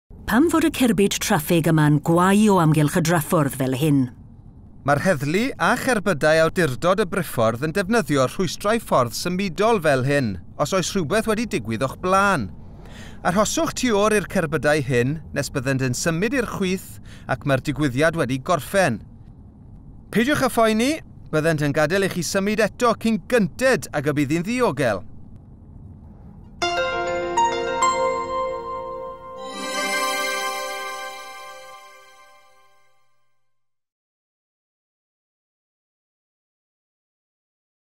Blue Light Aware Short 9 - Rhwystrau Rholio
Weithiau mae cerbydau'r Heddlu a'r Swyddog Traffig yn gwehyddu rhwng lonydd ar draffyrdd a cherbydau deuol, gan ffurfio "bloc ffordd dreigl." Yma rydyn ni'n dangos i chi sut y gallwch chi helpu.